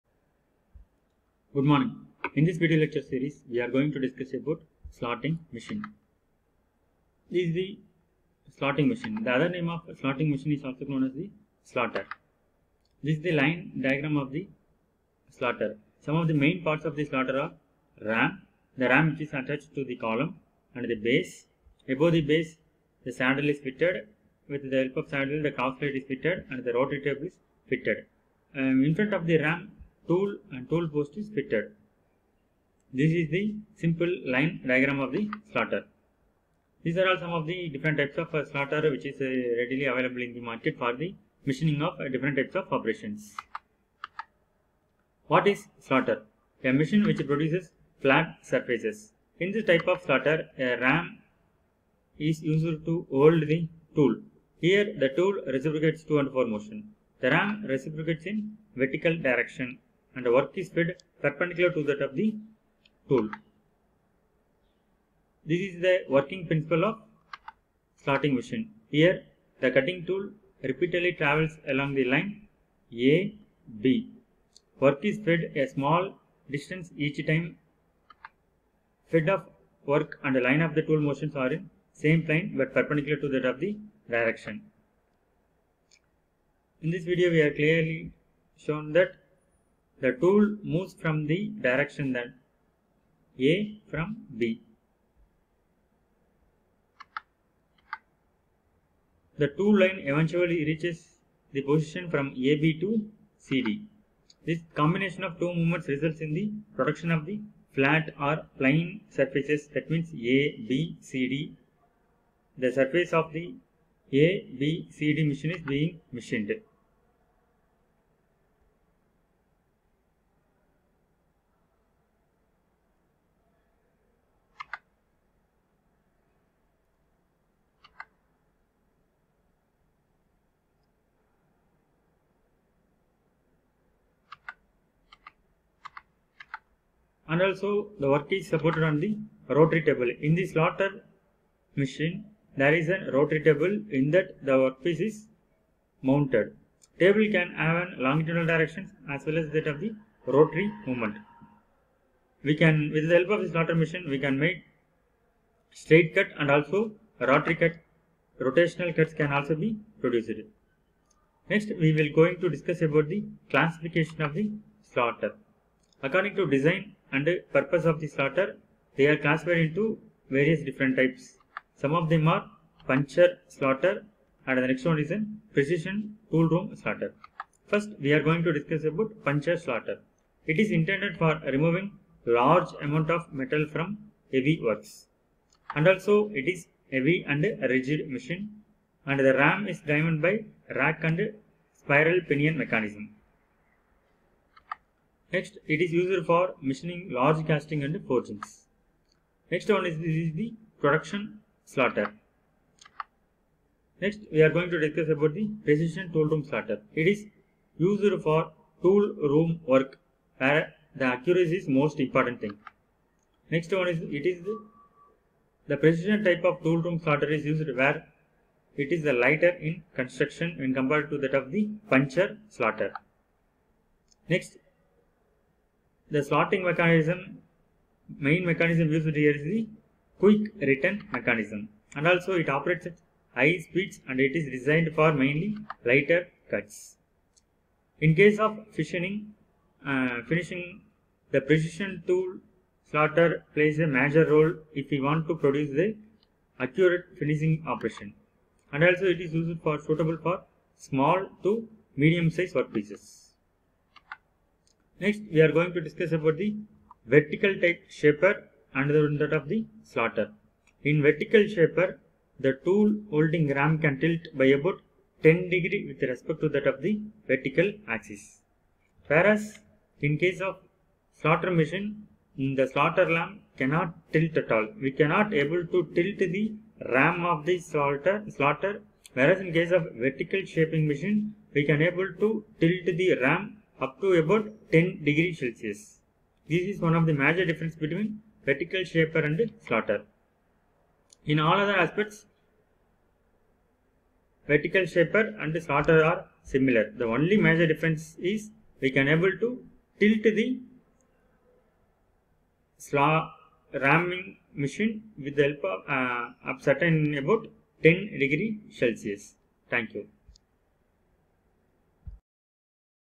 Slotter
The slotter machine is a precision-driven tool in metalworking and machining processes. It plays a crucial role in shaping and cutting slots, keyways, and intricate internal or external profiles on workpieces. Utilising a reciprocating ram and specialised cutting tools, slotter machines bring accuracy and precision to tasks that require the creation of slots and grooves in various materials.